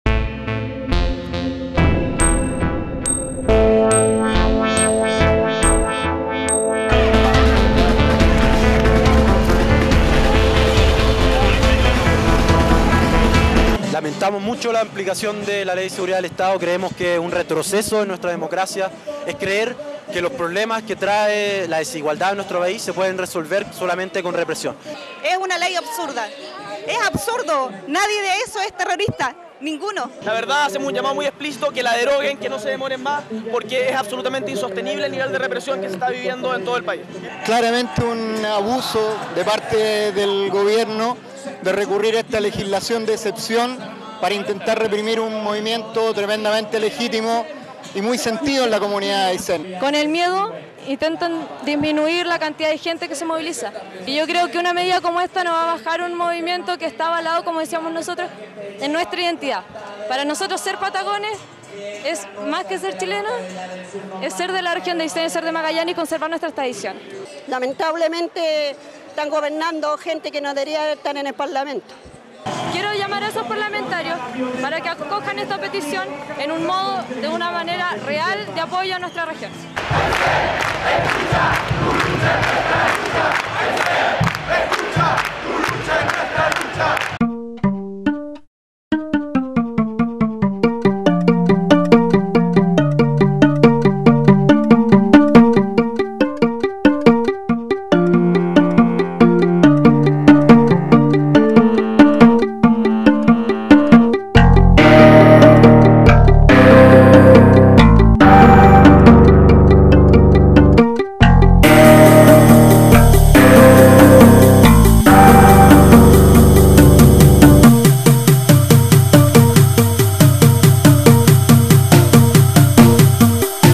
CHILE MARCHA CON AYSÉN
Exclusivo. OPAL. Corresponsal. Santiago de Chile. 21 Marzo 2012. En solidaridad con el pueblo de la región de Aysén y en rechazo a la represión y violencia policial que vive la zona. Policía reprime a manifestantes al terminar la jornada